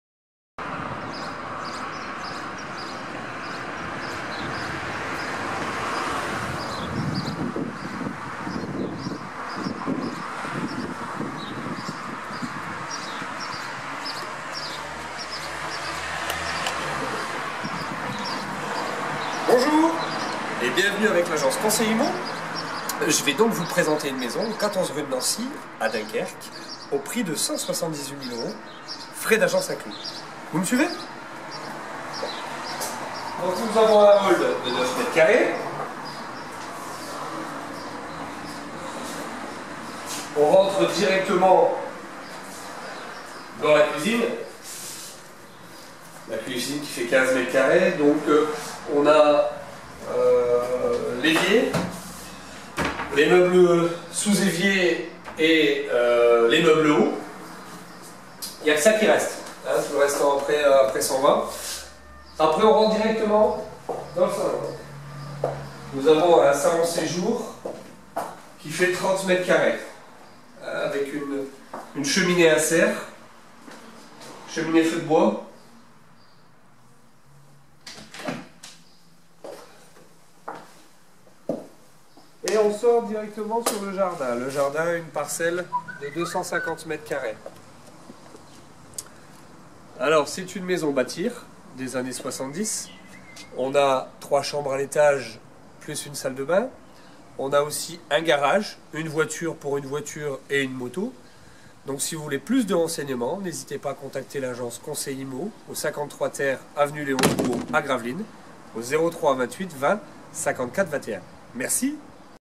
maison petite synthe
EXCLUSIVITÉ MAISON BÂTIR SUR DUNKERQUE 110 m2 COMPRENANT : Hall, salon, séjour, cuisine aménagée et équipée, salle de bains, 3 chambres, garage, jardin 350m².FAIRE VITE, COUP DE COEUR ASSURE ! MAISON TRÈS SOIGNÉE CONSEILIMMO 53 TER AVENUE LEON JOUHAUX 59820 GRAVELINES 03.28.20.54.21